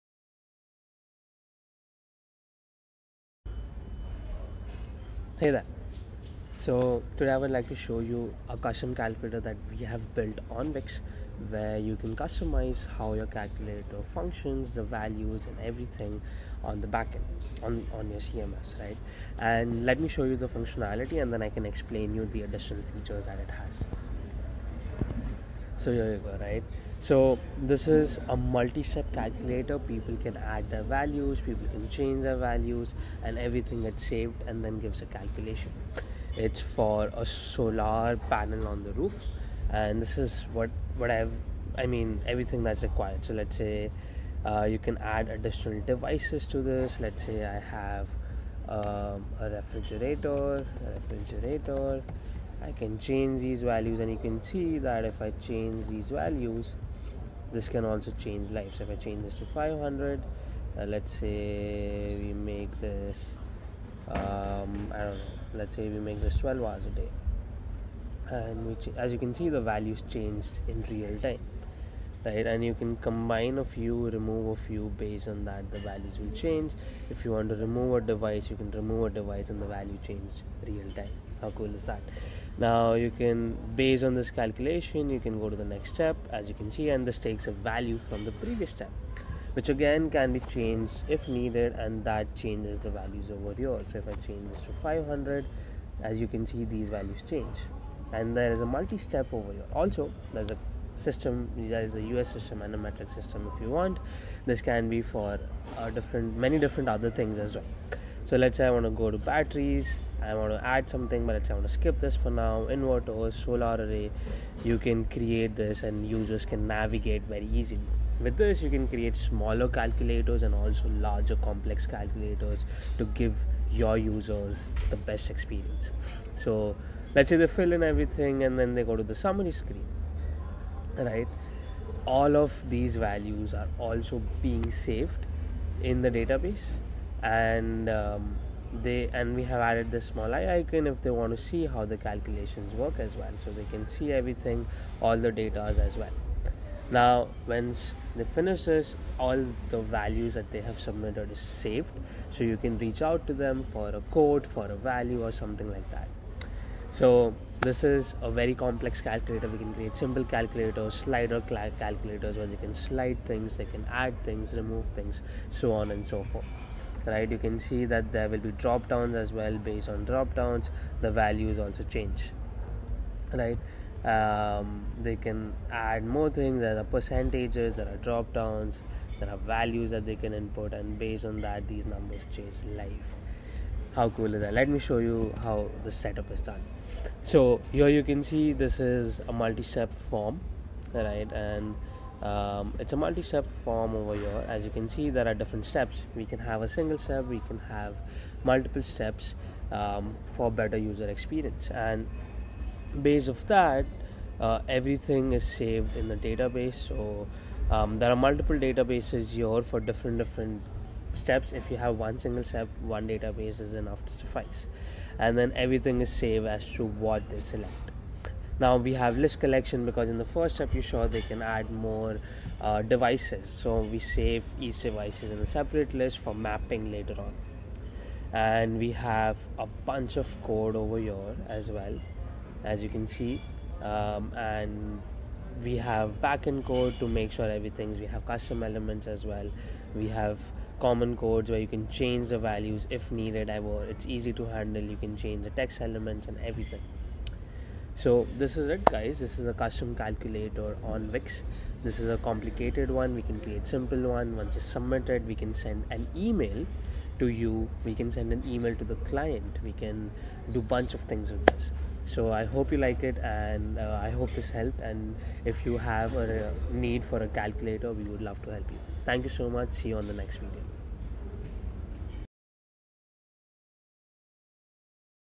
Custom Calculator on Wix | Wix Freaks | Wix Studio | Wix Velo
Take your website to the next level with a customizable multi-step calculator! This video will show you a Custom Calculator's Functionality with multiple inputs!

In this video, you will learn how about-

- Custom fields: Collect user input for your specific calculations.
- Set up formulas: Make your calculator perform complex functions.
- Display results clearly: Present calculations in an easy-to-understand format.

You can use this calculator if you have an e-commerce, healthcare, finance, and investment website and need to calculate things like shipping costs, BMIs, potential returns estimate and so much more!